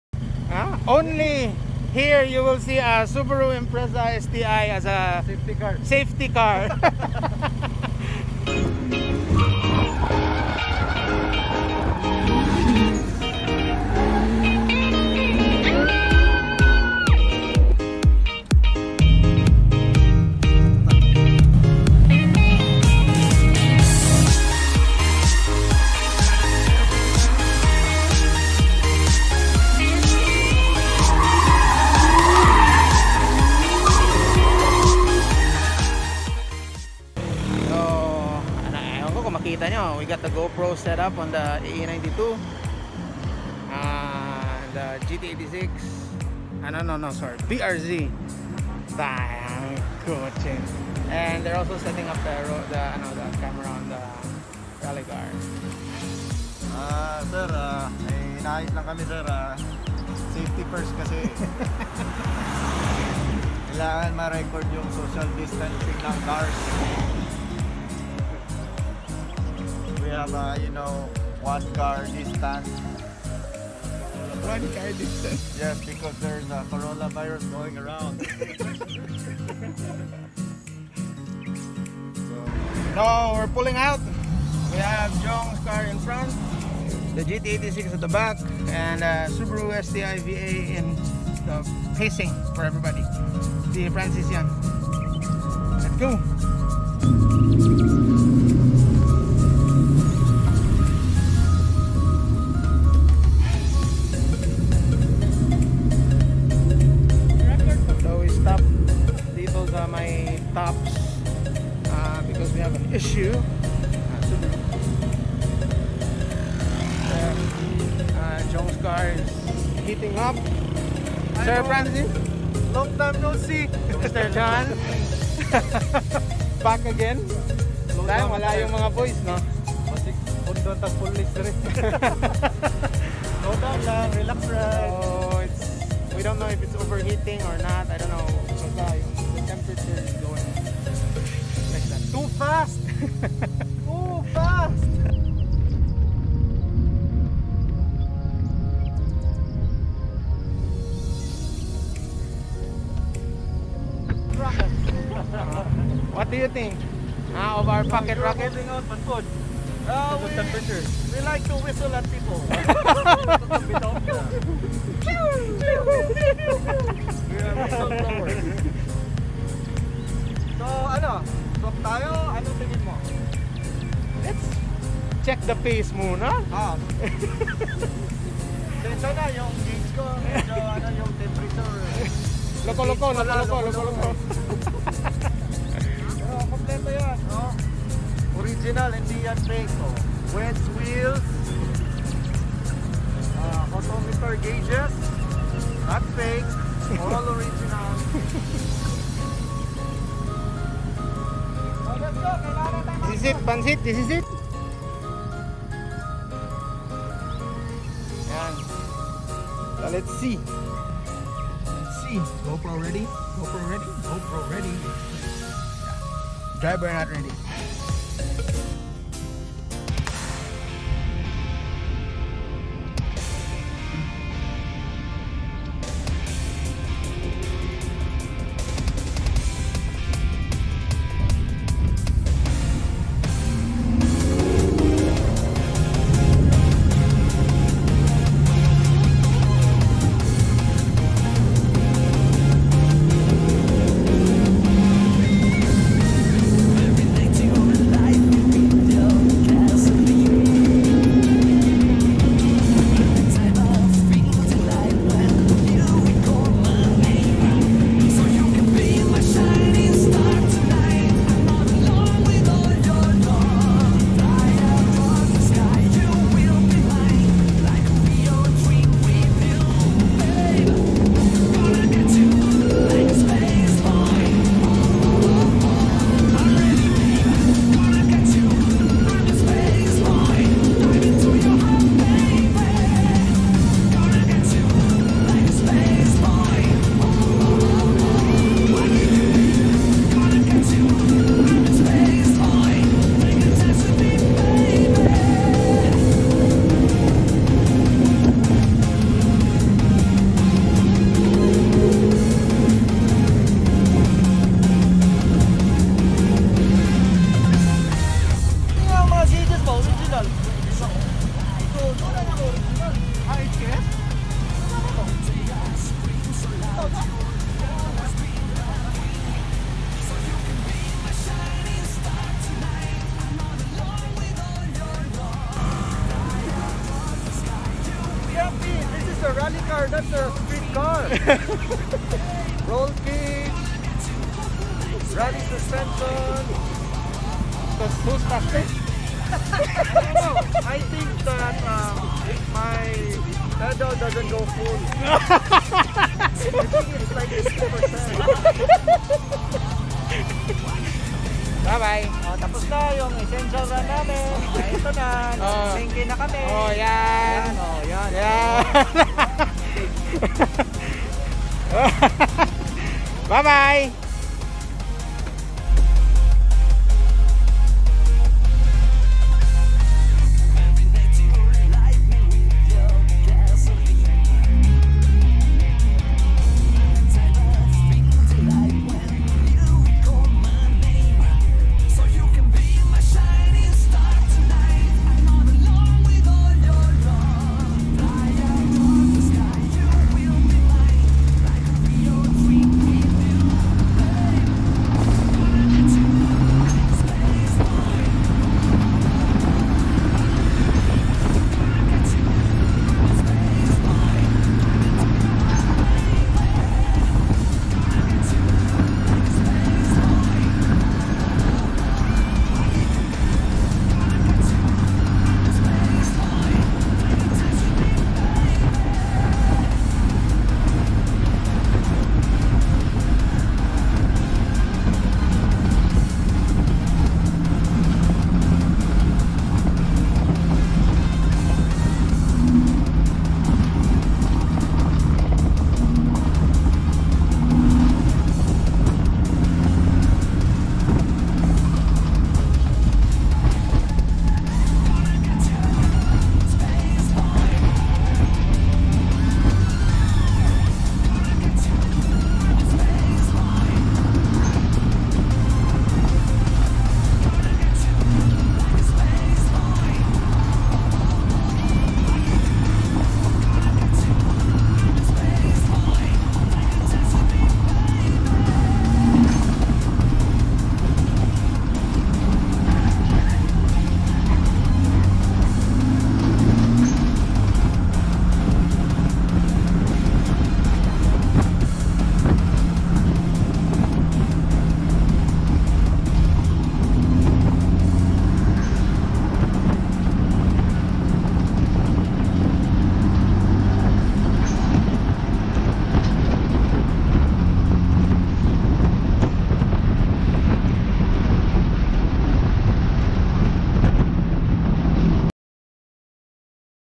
Touge Battle AE92 vs G100 - Vlog
Battling the mountain pass of Cebu, Philippines against my mentor and friend Jong King on his 4E-FTE Powered Daihatsu Charade G100